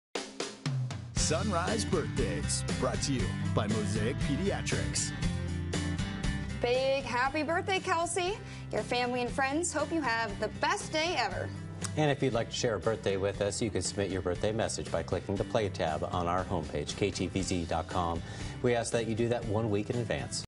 Sunrise Birthdays - Wednesday, May 17th
Here are NewsChannel 21's Sunrise Birthdays for Wednesday, May 17th. You can see more recent entries and submit an upcoming birthday to air